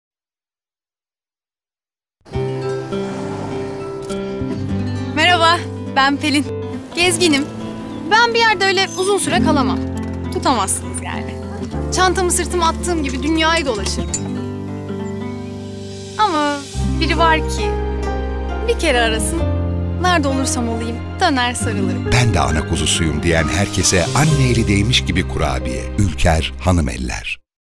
ÜLKER HANIMELLER REKLAM FİLMİ OYUNCUM RAJESH RAPAKAYA SONSUZ TEŞEKKÜRLER
CANIM  HİNTLİ OYUNCUMRAJESH RAPAKAY LA YAPTIĞIMIZ GÜZEL BİR ÇALIŞMAMIZ...